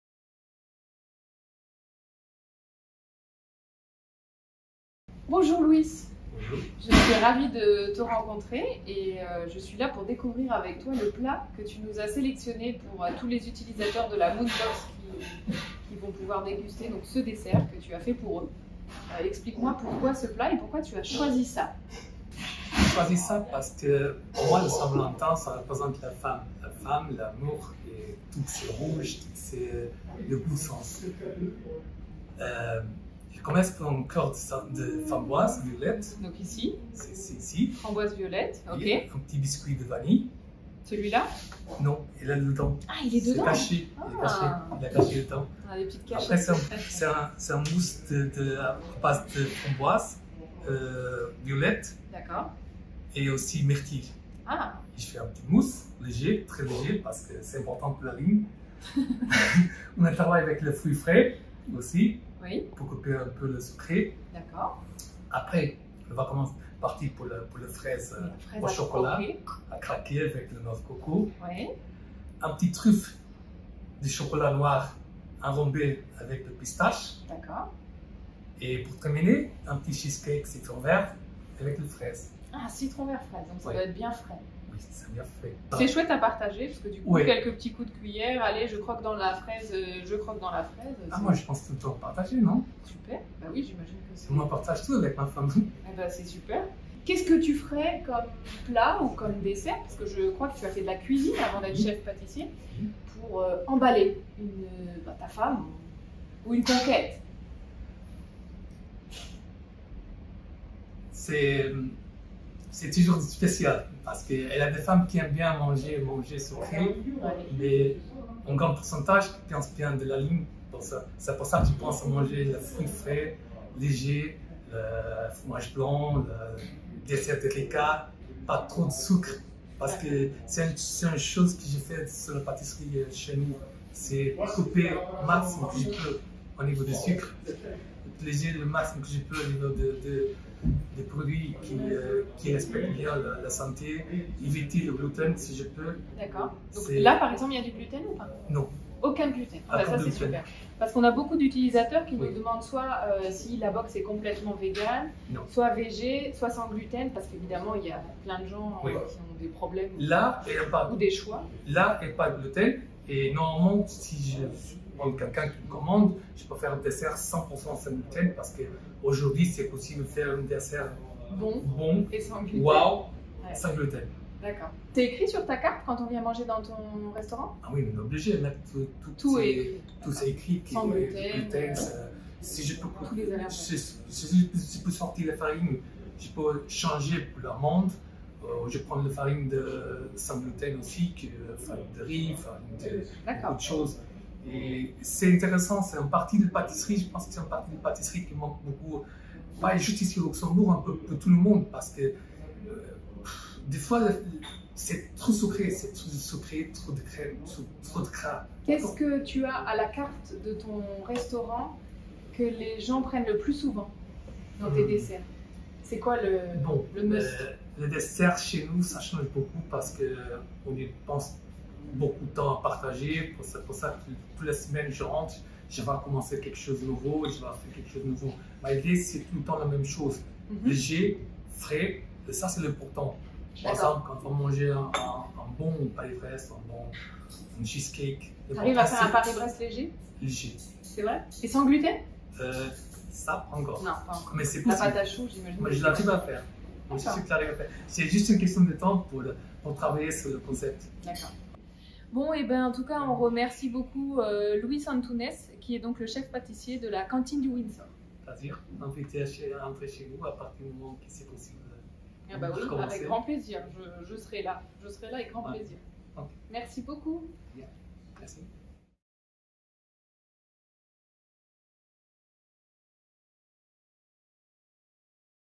Luis Antunes - Cantine Windsor
Chef Luis Antunes nous parle de lui et du plat qu'il a choisi pour notre MOON Box de la Saint-Valentin